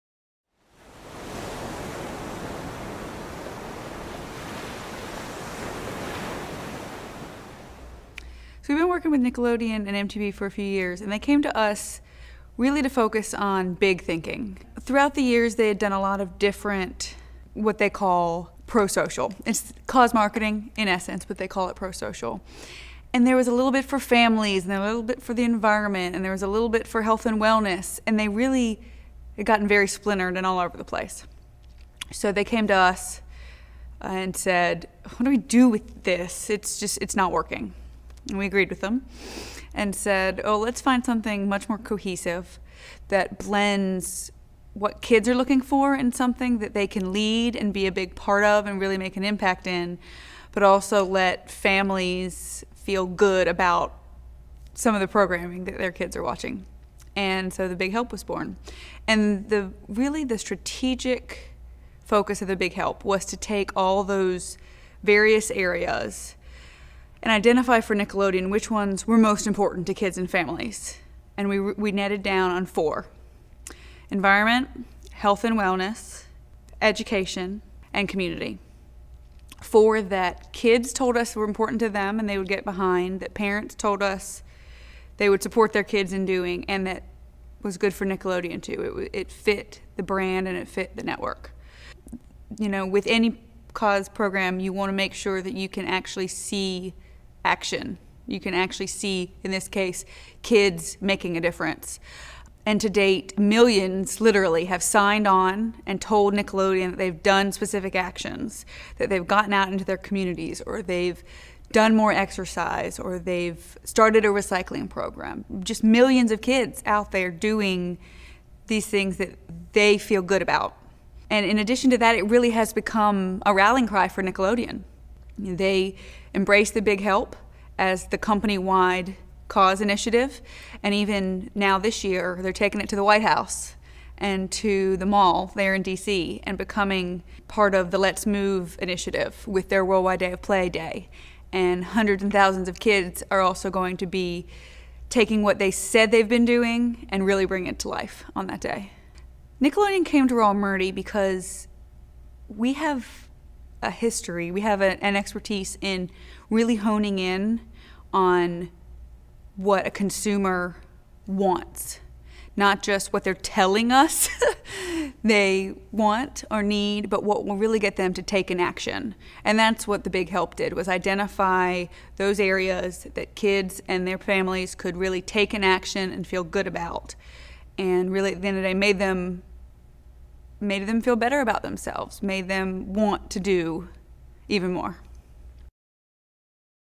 Nickelodeon's BIg Help Case Study
Through a strategic restructuring of pro-social efforts, Rawle Murdy helped Nickelodeon unify its many philanthropic causes and inspire kids to give back to their communities.